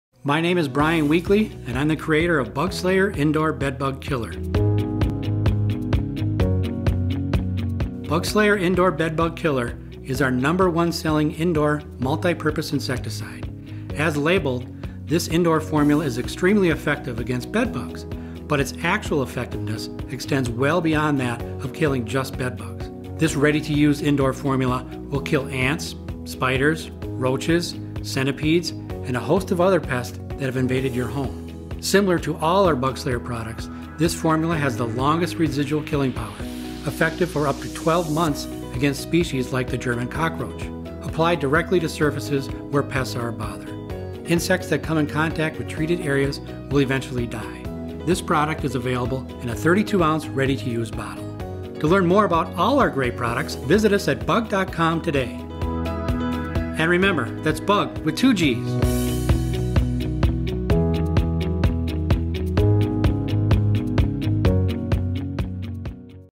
BUGGSLAYER insecticide INDOOR BED BUG killer + 50 other insects
BUGGSLAYER indoor Bed Bug Killer is our number one selling indoor multi-purpose insecticide.

As labeled, this indoor formula is extremely effective against Bed Bugs - but its actual effectiveness extends well beyond that of killing just Bed Bugs.  This ready-to-use indoor formula will kill ants, spiders, roaches, centipedes and a host of many other pests that have invaded your home.

Similar to all of our BUGGSLAYER products, this formula has the longest residual killing power, effective for up to 12 months against species like the German cockroach.  Apply directly to surface areas where pest are a bother.  Insects that come in contact with treated areas will eventually
die.

BUGGSLAYER KILLS:
• Asian lady beetles
• Ants
• Armyworms
• Bees
• Birch catkin feeders
• Box elder bugs
• Centipedes
• Cluster flies
• Cockroaches
• Crickets
• Earwigs
• Fire ants
• Fleas
• Flies
• Gnats
• Grasshoppers
• Hackberry psyllids
• Hornets
• Japanese beetles
• Leaf-footed bugs
• Mosquitoes
• Palmetto bugs
• Scorpions
• Silverfish
• Spiders
• Springtails
• Stink bugs
• Ticks
• Wasps
• Waterbugs
• Western conifer
seed bugs
• Yellow jackets
• Plus many more
(listed insects)

To learn more about all of our great products, visit us at BUGG.com